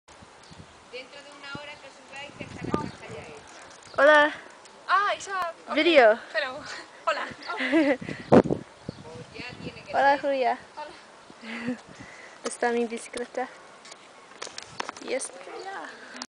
El Campo - Lago con las chicas
Con las Silvas en Caraquiz (?) , montando las bicicletas...